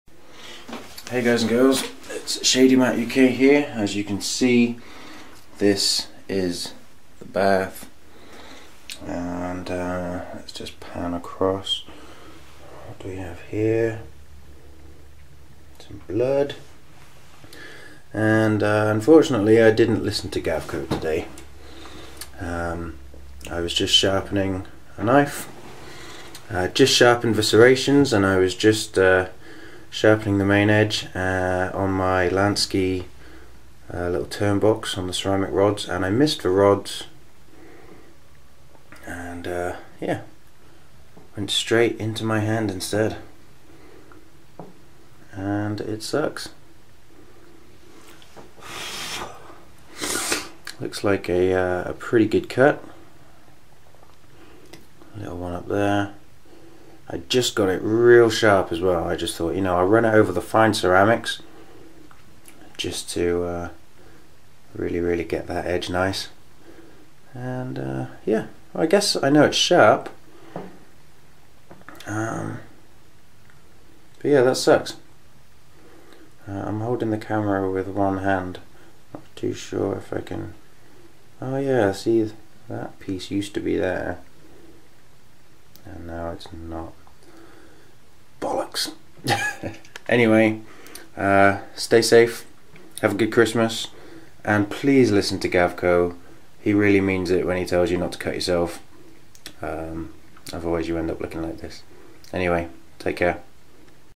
Nasty cut whilst sharpening a part serrated blade
Well that sucks!